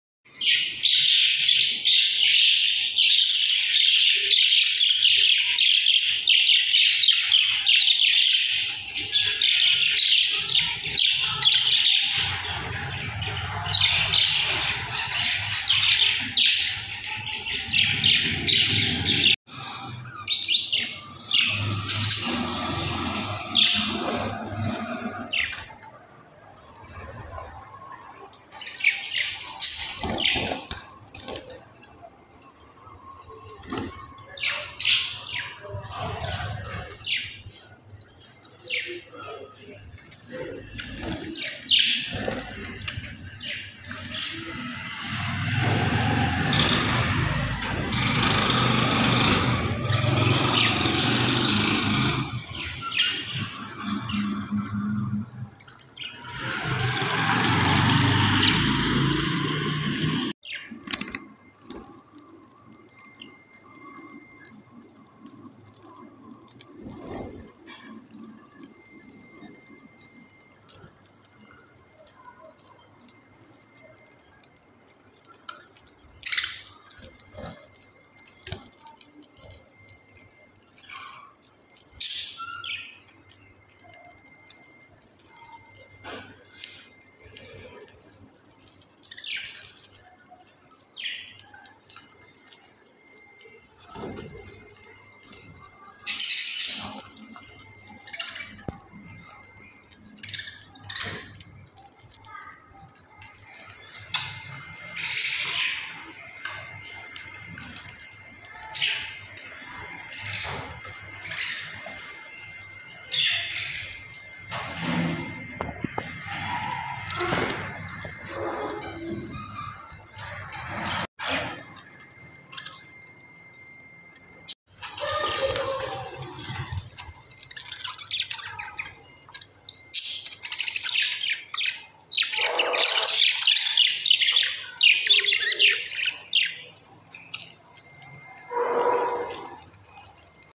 My Budgies Babies And Pets/ Budgies Enjoying And Eating Spinach
My Budgies Enjoying And Eating Spinach.
 Spinach also contains several other vitamins and minerals, including potassium, magnesium, and vitamins B6, B9, and E. Spinach is an extremely nutrient-rich vegetable.
 But twice a week should be enough for spinach as it is extremely high in iron and too much can cause iron toxicity.